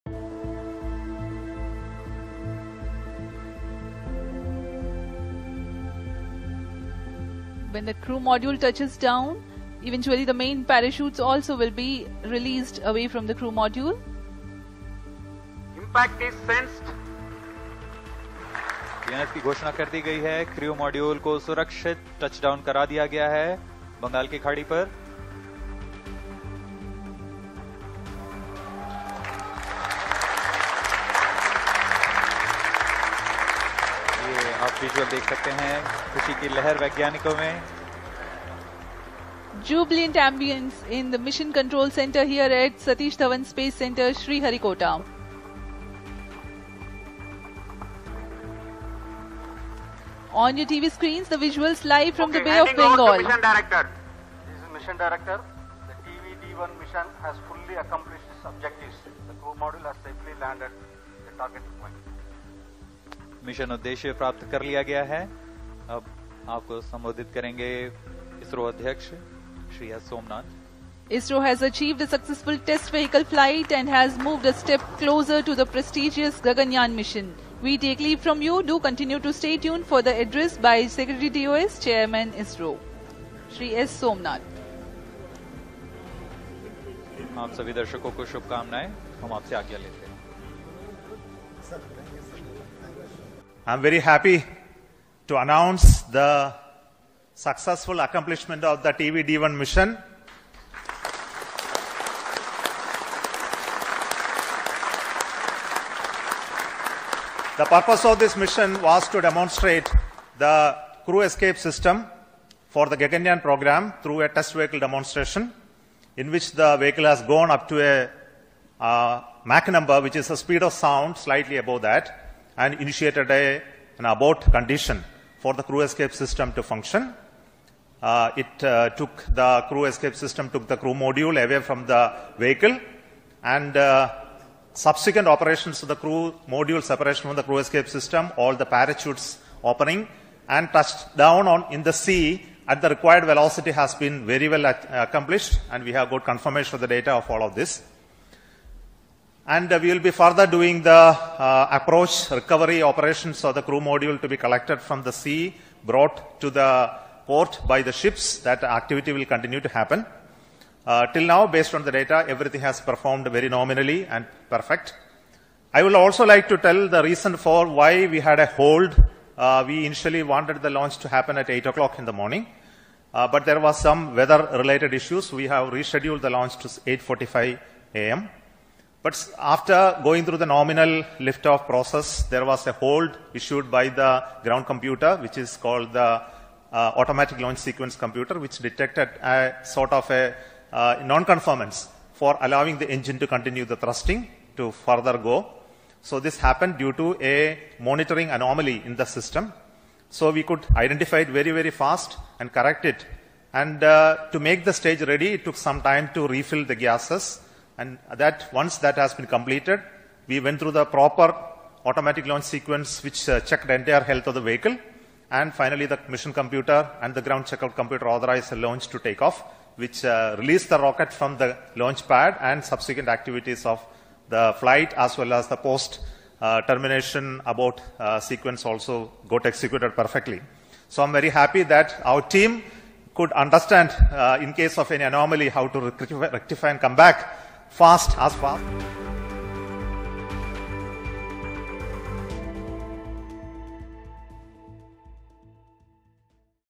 Gaganyaan Test Flight Successful, ISRO Crew Escape Model Splashes Down
Gaganyaan's test flight successfully tests the crew escape module. The module touched down in the Bay of Bengal - watch what the ISRO Chief had to say!